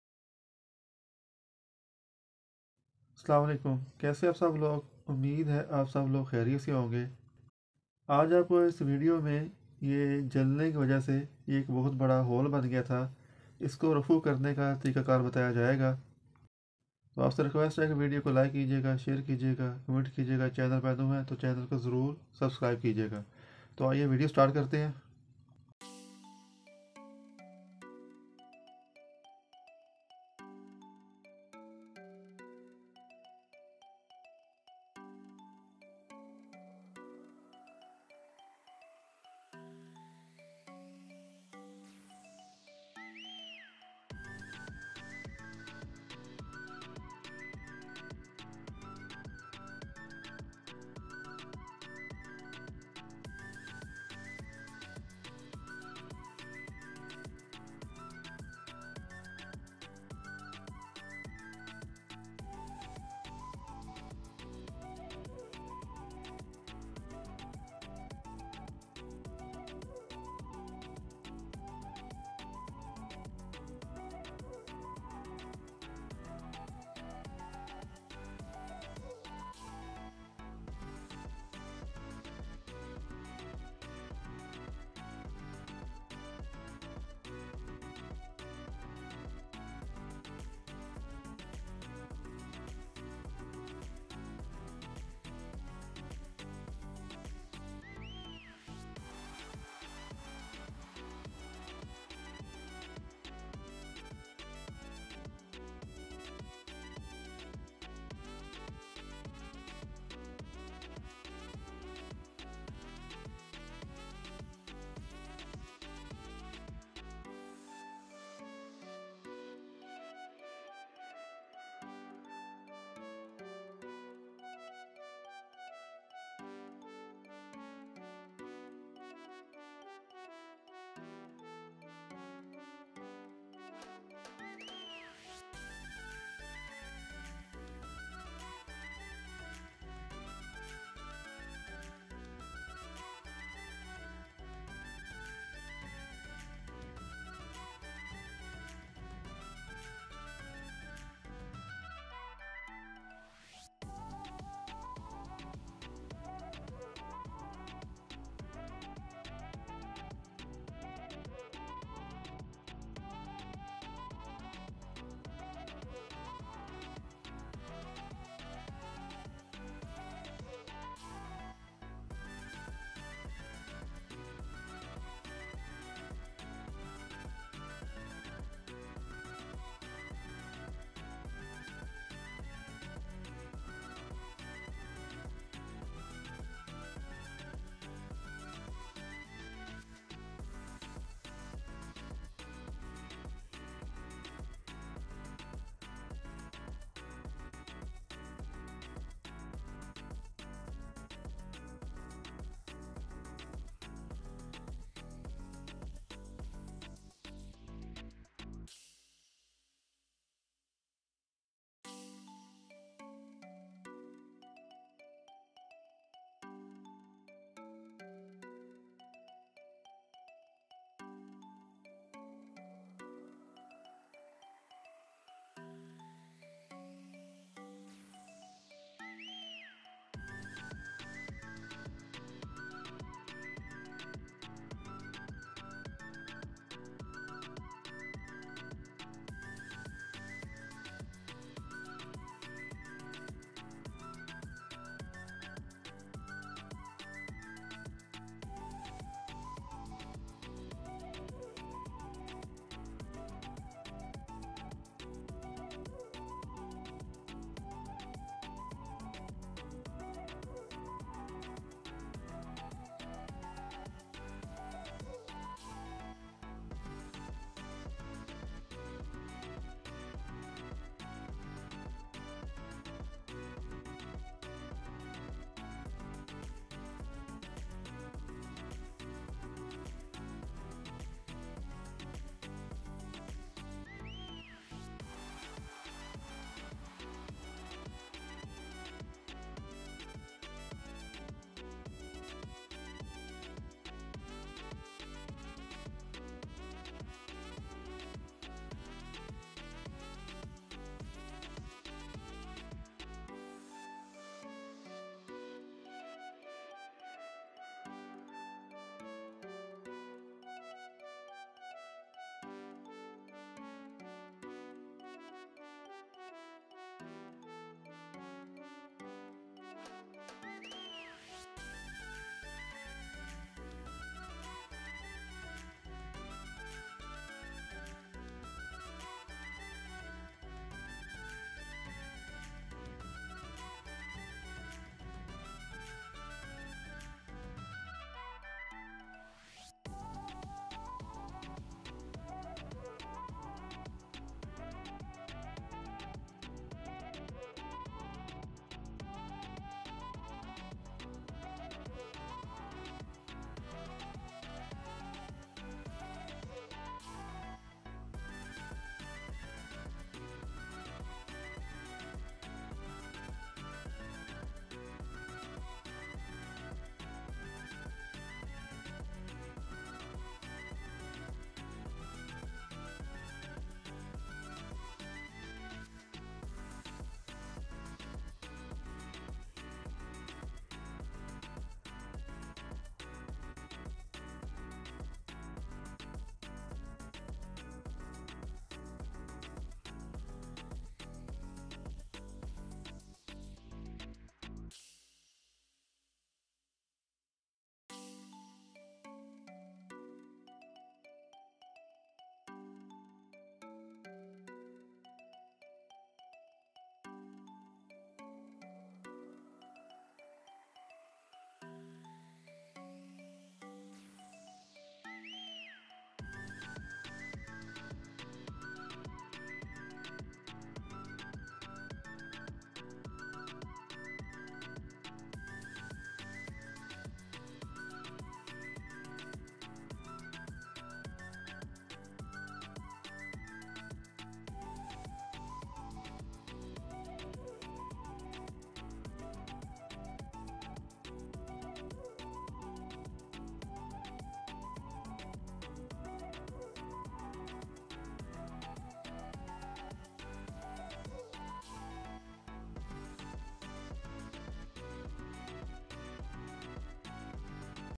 How To Sew A Hole On Your Kameez In A Wonderful And Elegant Way | Raffu Karne Ka Asan Tarika
How to sew a hole on your shirt in a wonderful and
elegant way using simple crafts to introduce sewing
lessons to teach hand sewing to beginners and
provide them with useful sewing techniques and new
and innovative sewing ideas to repair a hole on your
clothes using a needle and thread to sew the hole
from the outside where the inside is reinforced with a
very sticky cloth So that it is very strong when you
reuse it again.